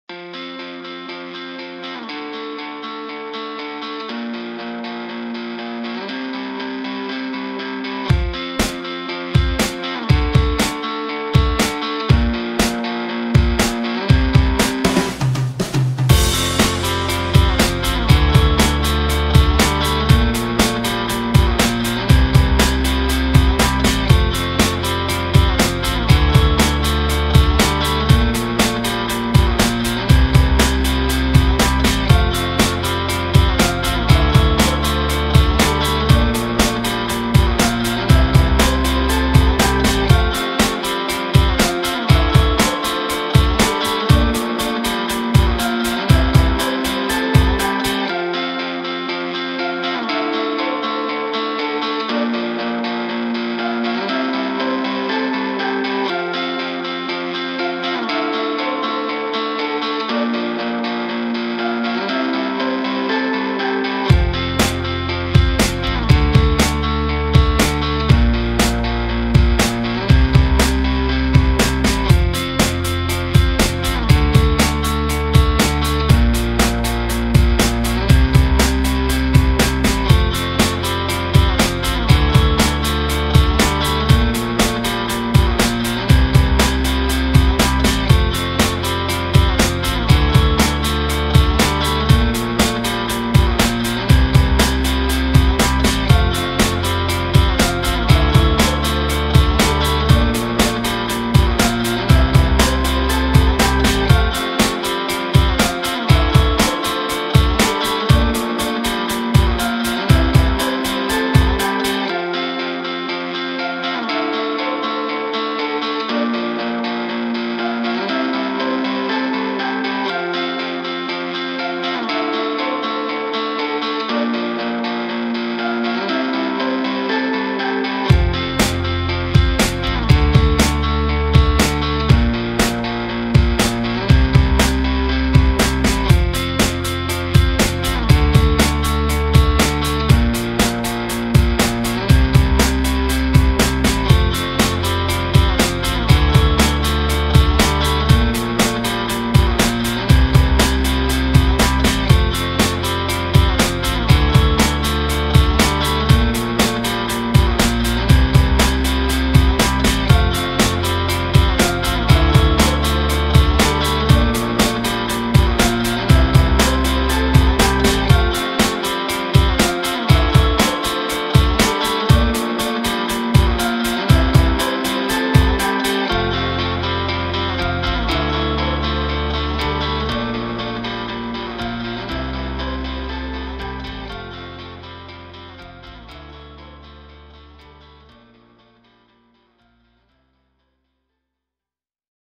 FREE Indie Rock Instrumental (The Strokes, The Killers Type) | Prod. DJ KHALED'S SON + Guala Beatz
Free use for nonprofit, must credit "DJ Khaled's Son & Guala Beatz"